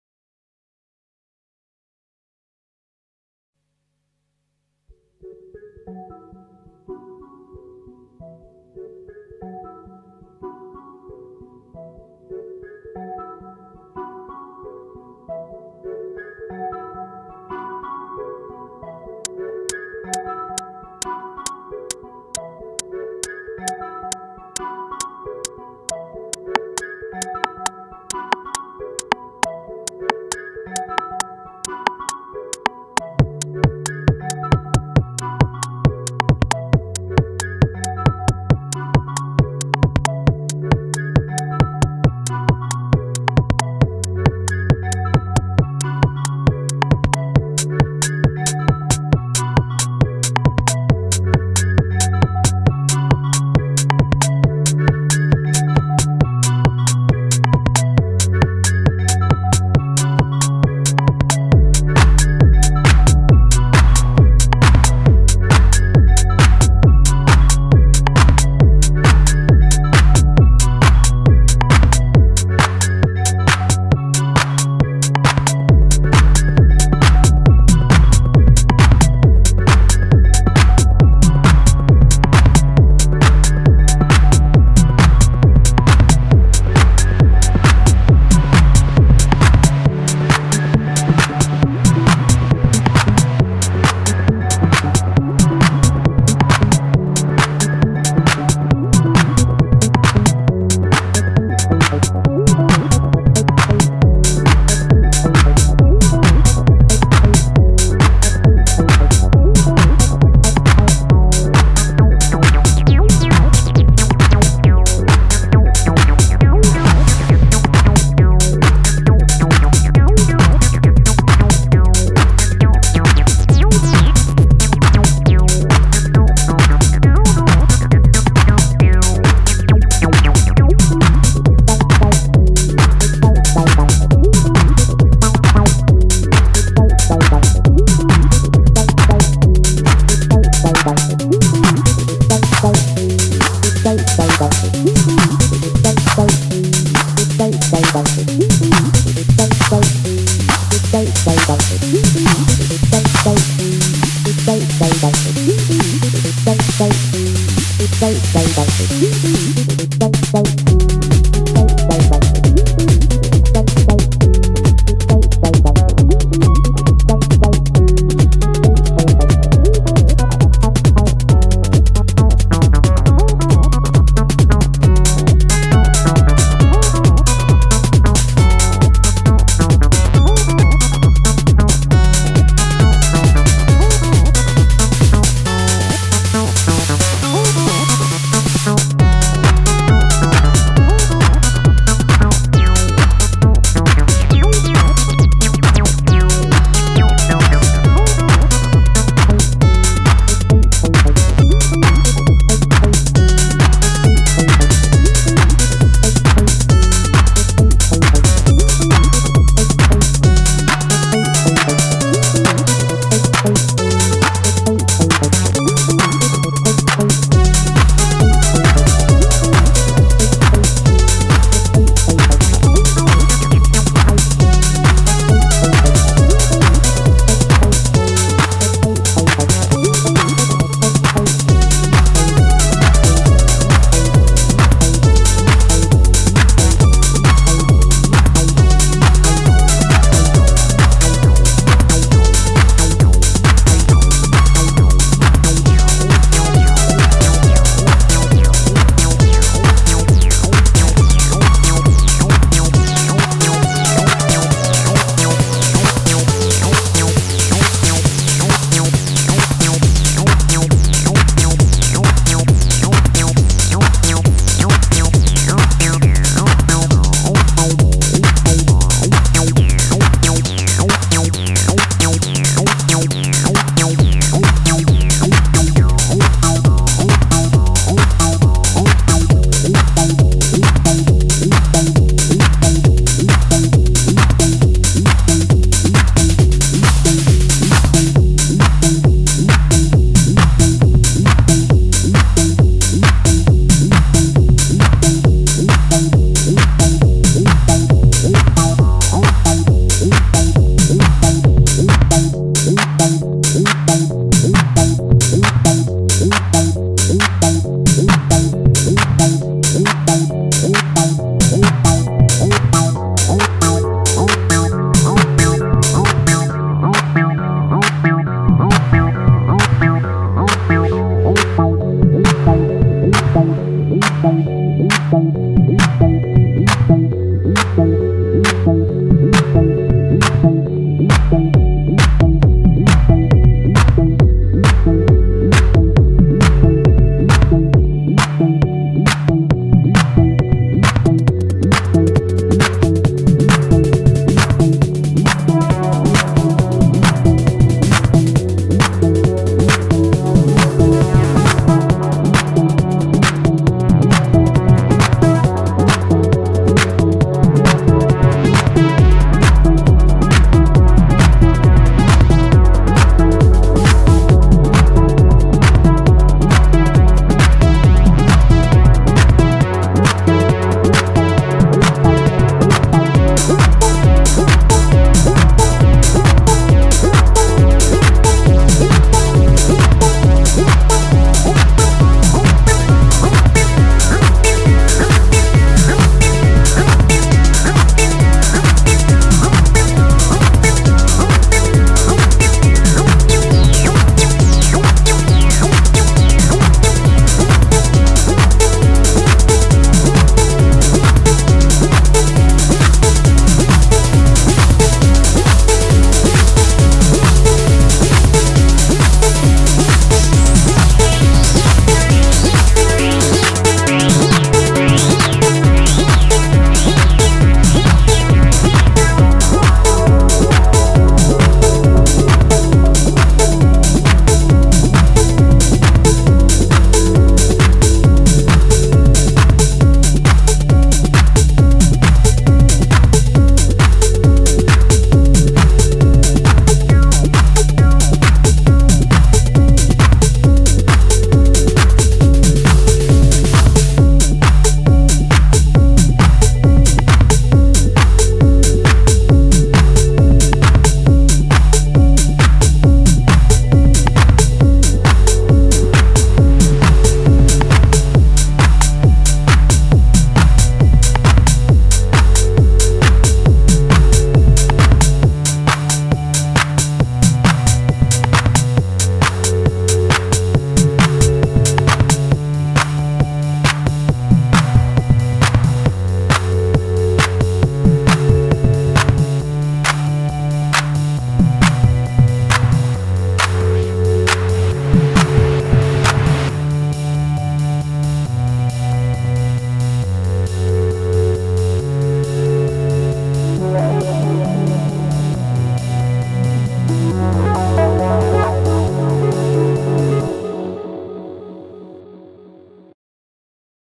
♥ Acid house ♥ Live Hardware ♥ Louis Gentiment - acid jam
Zaquencer / Tanzmaus / Volca / Poly800 /X0XB0X / Acid

So basically, my master clock is the TANZMAUS (drum machine) I use her internal sequencer (32step patterns) and send the clock to :
= The x0xb0X (black box to the left) wich do the acid lines, I use her internal sequencer (and used 4 patterns, the first one is 2pattern chained together)then the clock goes to :

= the BCR2000 (the big black box with LCD display) which has been hacked an now it's a 4 tracks sequencer (called zaquencer), it sends pattern to 3 machines :

= The volca keys,

=the monotribe (which do the boring bassline);

= The EX800 (poly 800 in a rack, that's the box under the monotribe) ; he is modified with the automahawk and hawk 800, it does the first lead and the pad at the end :)

Then everything played in sync and all I do is trig patterns, turn cutoff and volume knobs, activate some features on the synth ect ;